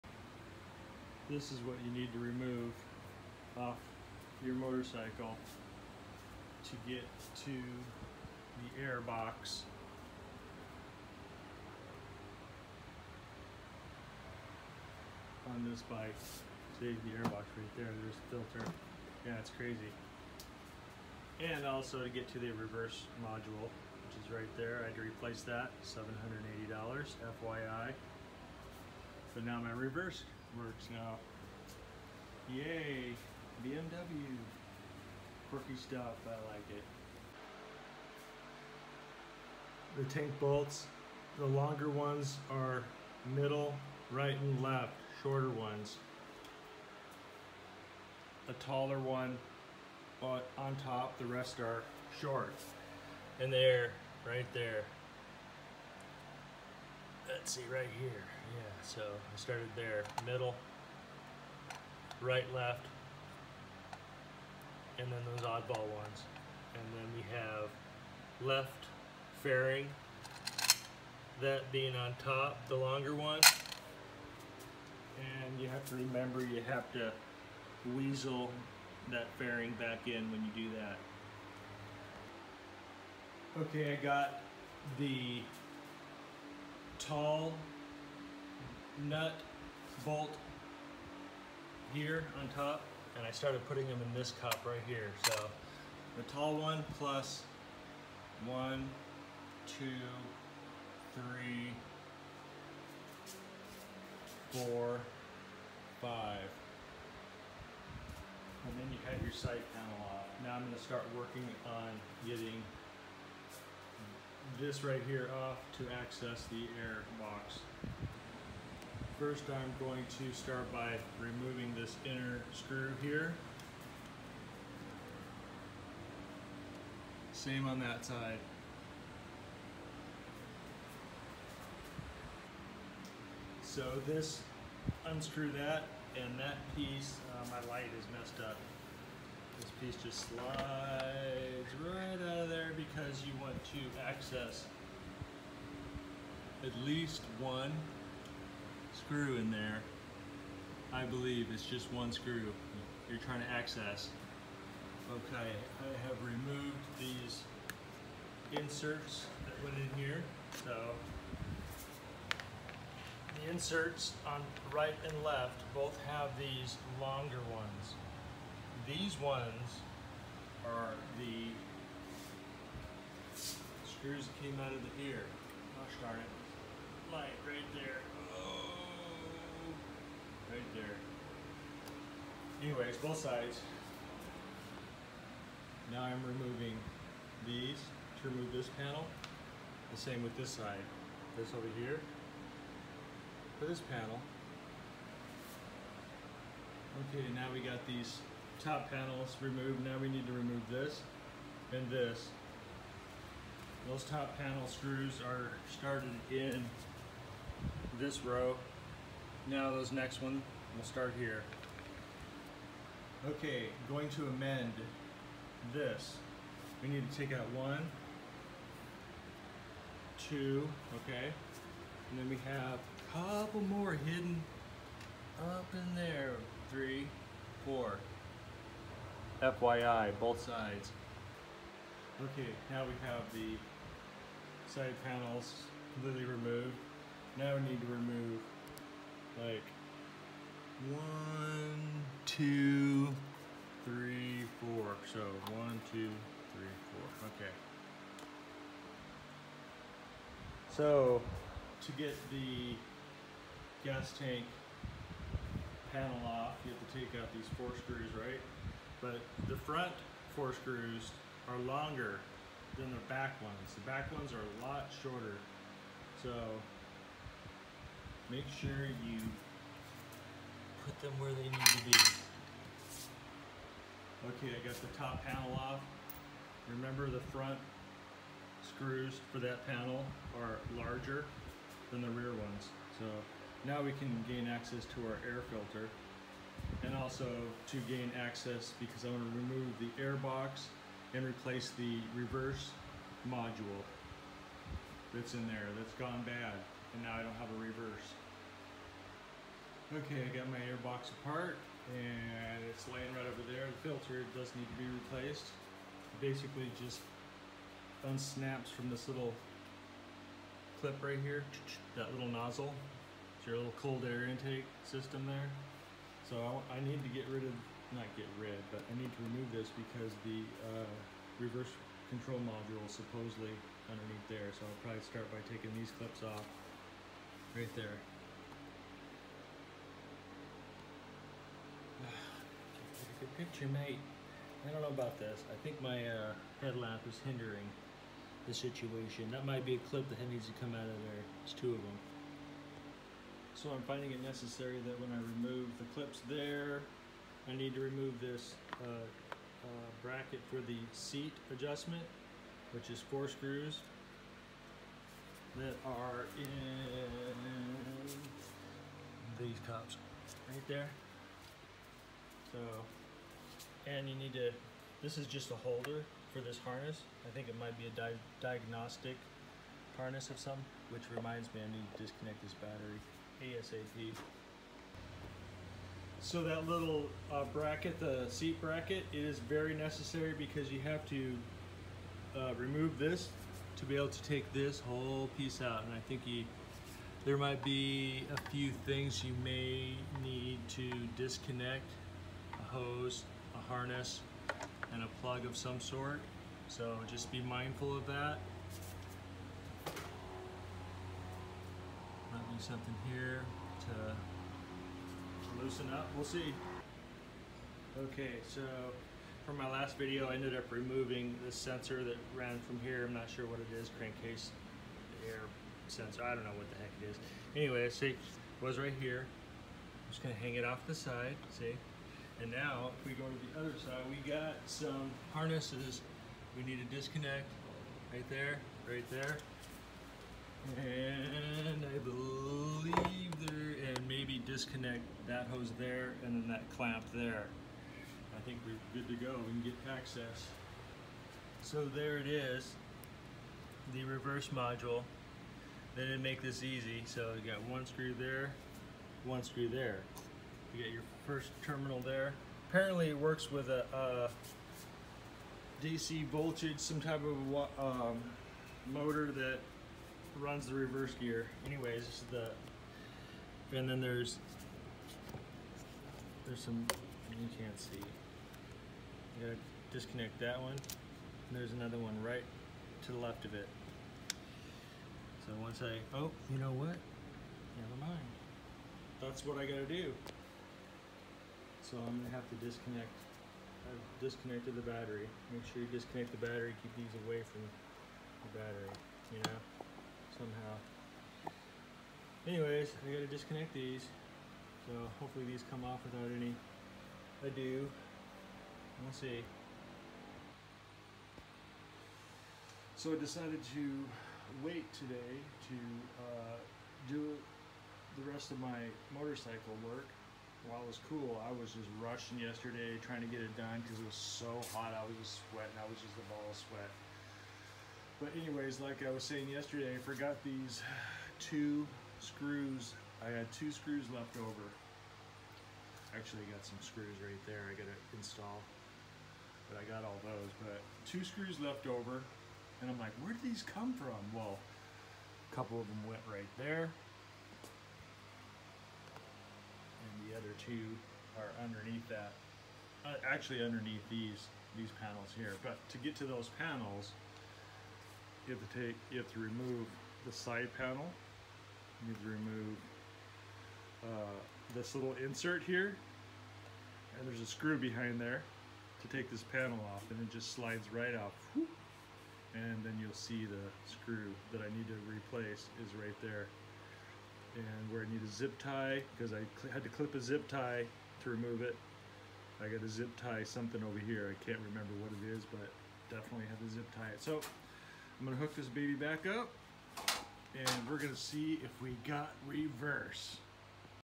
2018 BMW K1600GTL REVERSE CONTROL MODULE
I finally bit the bullet and bought my $810 part, since I could not get the part from Europe, half-price, and a bite here in the states, what a rip off, but at least my reverse works. not that I use my reverse gear a lot, but I have used it on occasions where I am in situations and gravel and what not.